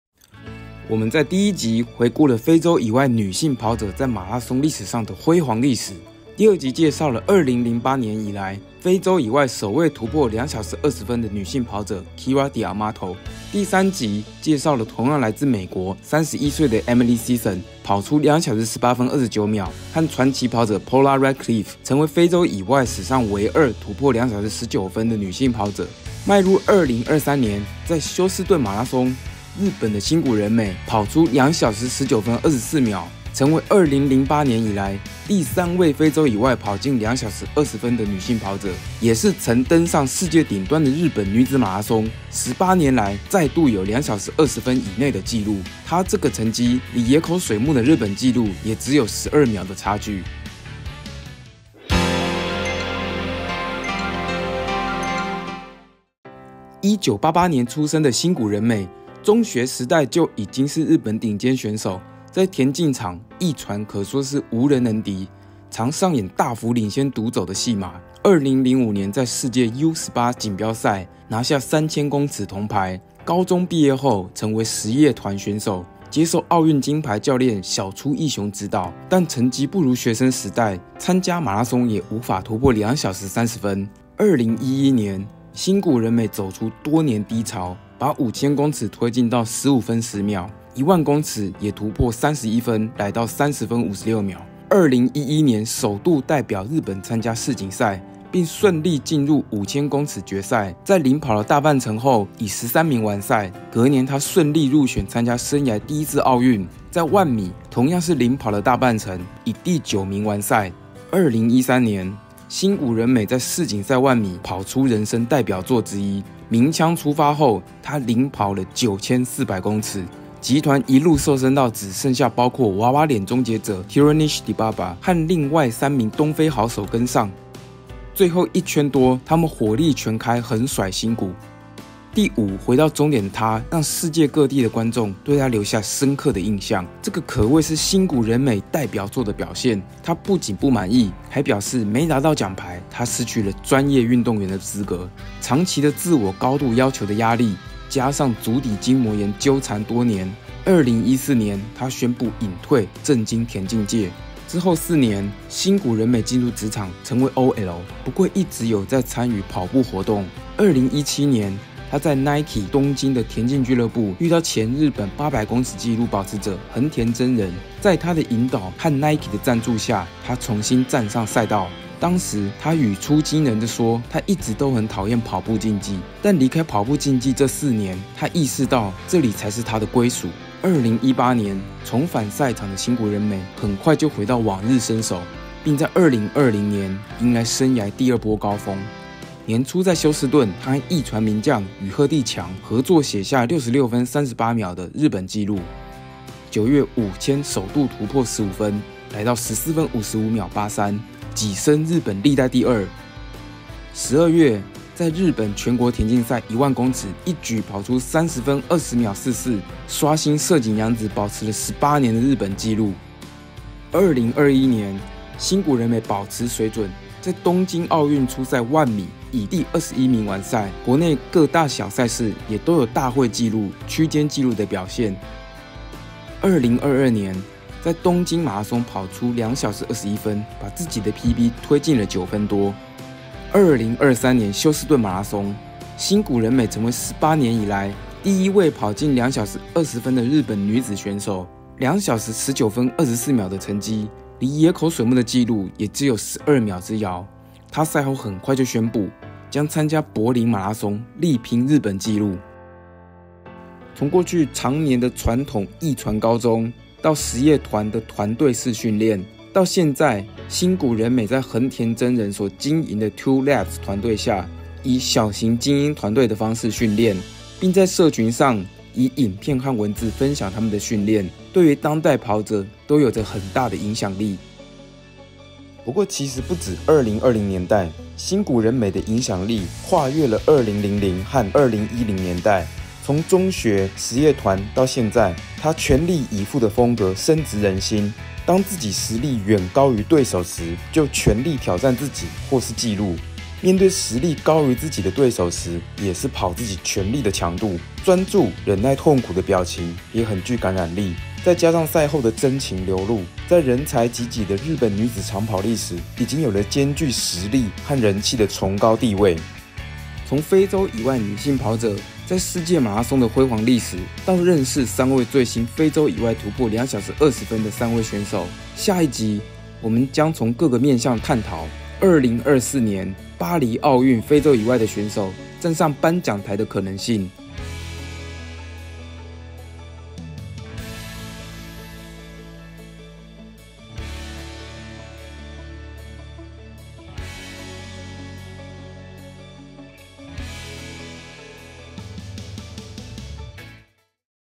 日本馬拉松新希望 2:19:24 新谷仁美 | 歐亞美女子馬拉松榮光再起 EP4
2023年休士頓馬拉松，新谷仁美成為18年來第一位跑進 2:20 的日本女子選手，2:19:24 這個成績，離野口水木的紀錄也只有12秒之遙！

從過去長年的傳統驛傳高中、實業團的團隊式訓練，到現在新谷仁美在橫田真人所經營的「TWOLAPS」團隊的小型菁英團隊的方式訓練，新谷仁美的影響力跨越了2000到2020年代，全力以赴的風格深植人心，無論對手實力如何，她都全力挑戰自己或是紀錄的強度，專注、忍耐痛苦的表情、賽後的真情流露，在日本女子長跑歷史已有崇高地位。

2:18:29 非洲以外史上第二 Emily Sisson | 歐亞美女子馬拉松榮光再起 EP3

史上最虐賽道 巴黎奧運 歐美亞跑者能殺出重圍嗎 | 歐亞美女子馬拉松榮光再起 EP.5